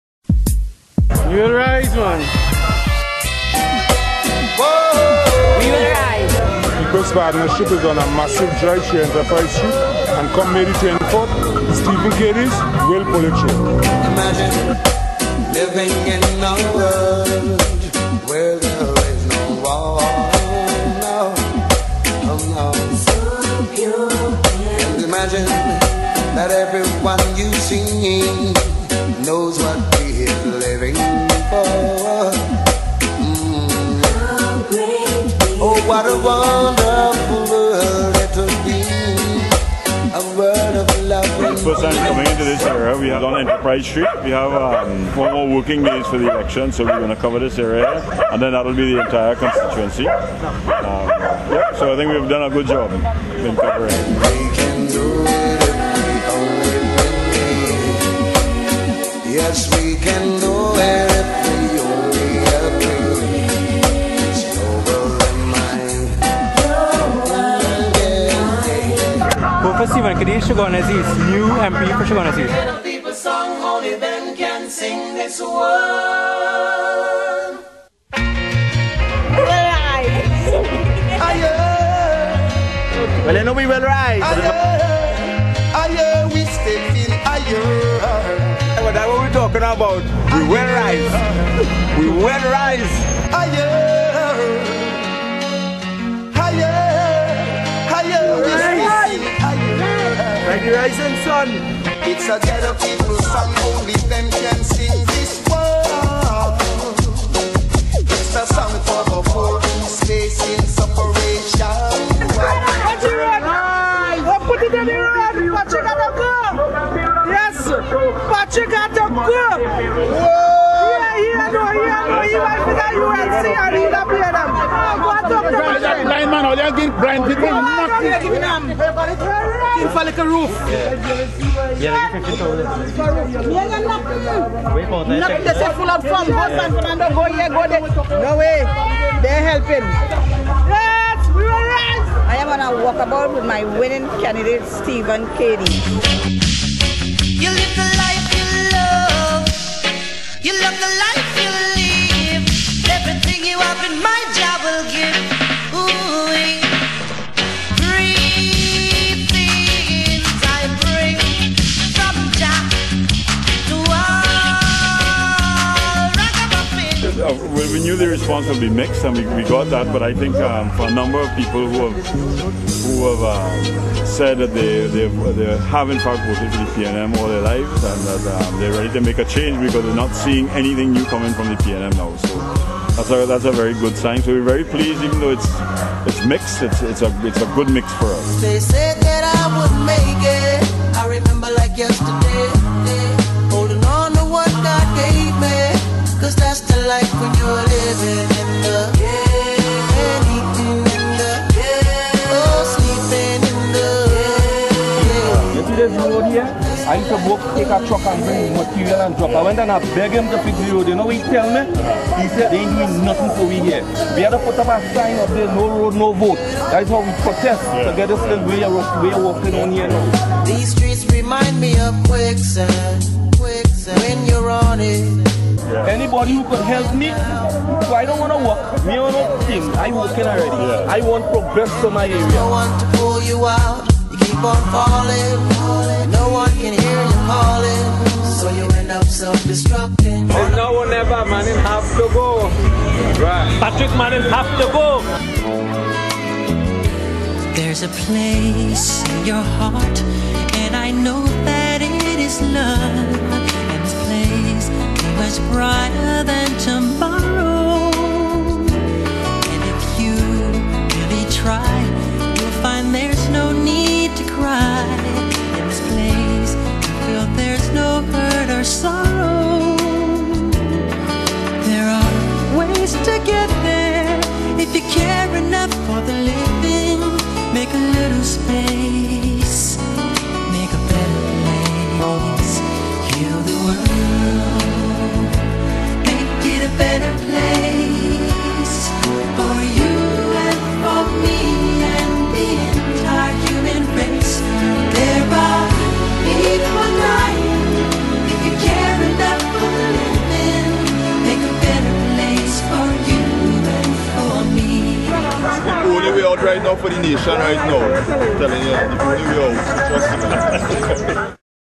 Stephen Cadiz _ Keepin it Real Walkabout.wmv
Join your United National Congress Candidate for Chaguanas East as he walks through the communities of Enterprise and Longdenville...listening to the plights of his constituents.